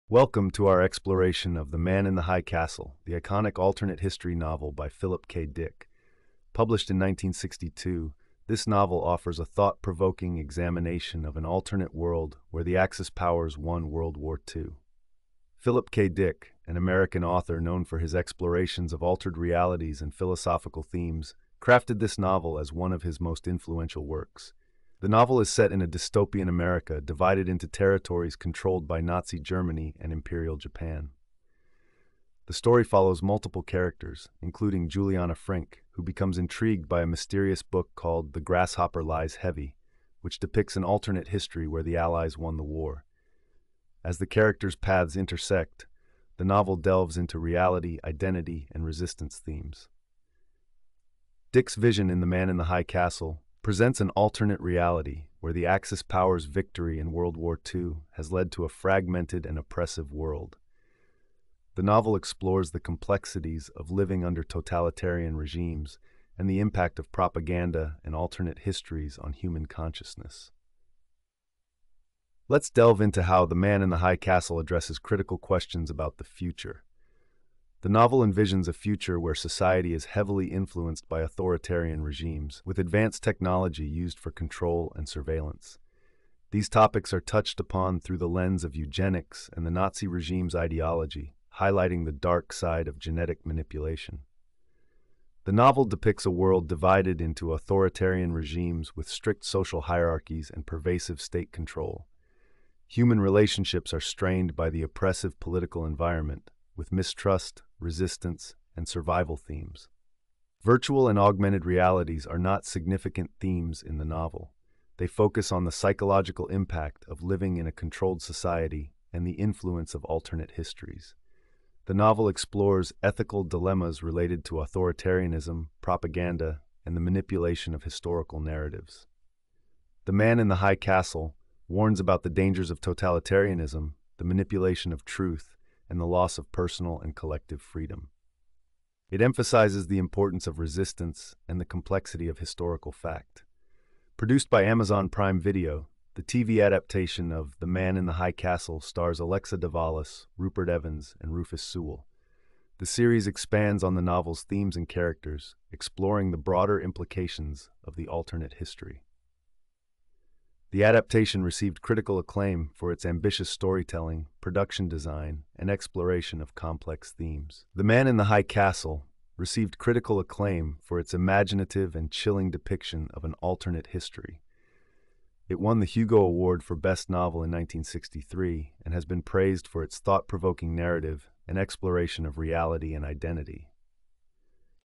Man in the High Castle: A Very Real Story (2024)
Join us on a journey through alternate realities as we delve into the intriguing world of "The Man in the High Castle" by Philip K. Dick. Explore the thought-provoking themes, complex characters, and gripping storylines that make this novel a must-read for fans of science fiction and alternate history.

Content
0:00 Introduction
0:02 Overview of the Novel
0:11 Philip K. Dick and His Vision
0:16 Setting: Dystopian America
0:22 Main Characters and Plot
0:28 The Grasshopper Lies Heavy
0:36 Themes of Reality, Identity, and Resistance
0:52 Totalitarian Regimes and Their Impact
1:13 Future Envisioned by the Novel
1:24 Control and Surveillance
1:32 Eugenics and Genetic Manipulation
1:36 Authoritarian Regimes and Social Hierarchies
1:48 Psychological Impact of Controlled Society
2:03 Ethical Dilemmas and Historical Narratives
2:11 Warnings Against Totalitarianism
2:17 Importance of Resistance
2:24 TV Adaptation by Amazon Prime
2:36 Critical Acclaim and Awards